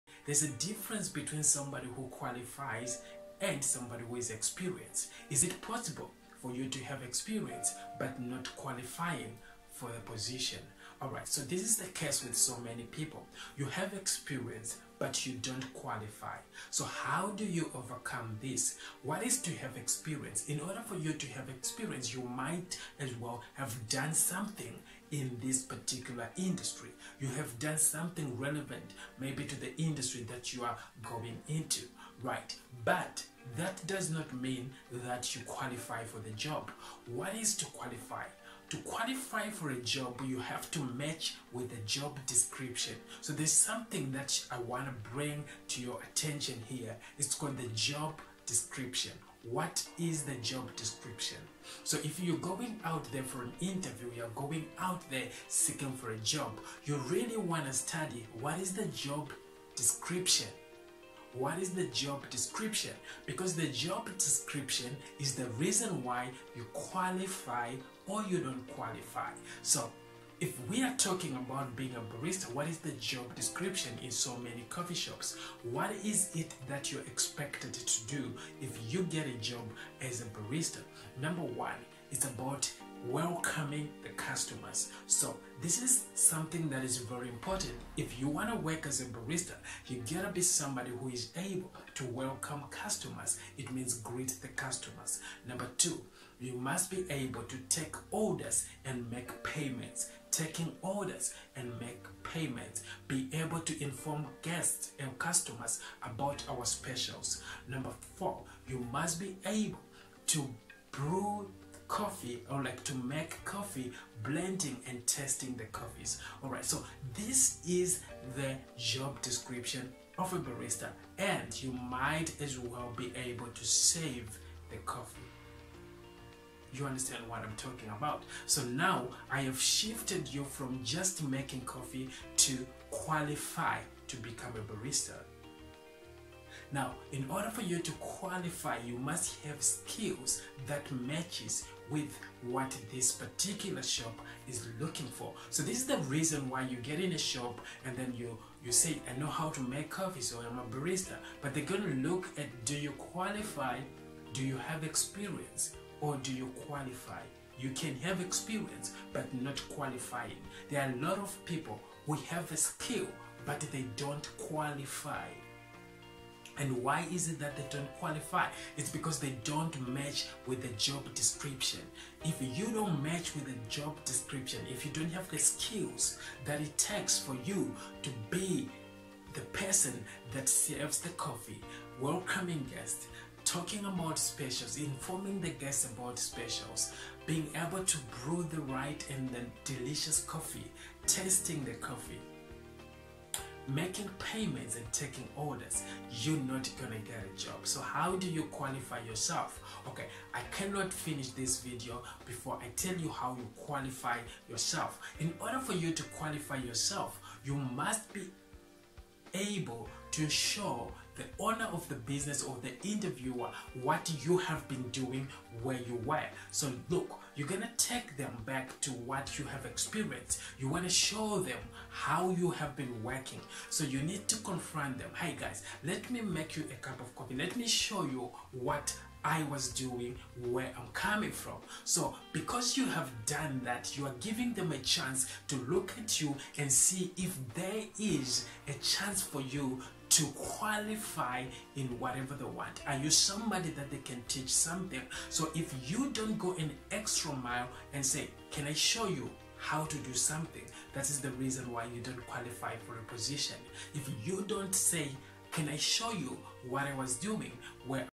How to quilfy yourself for a job
A job description is a statement that outlines the specifics of a particular job or position with a company. It goes into detail about the responsibilities and conditions of the job.

The question is.. DO YOU QUILFY?  You can have work experience but not qualifying for the job..
How do you qualify yourself?
1. Know the job description
2.show quilities that match their needs
3.go an extra mile to showcase your skills and experience..

@kudzaicoffeecompany join the training and coaching to get a job...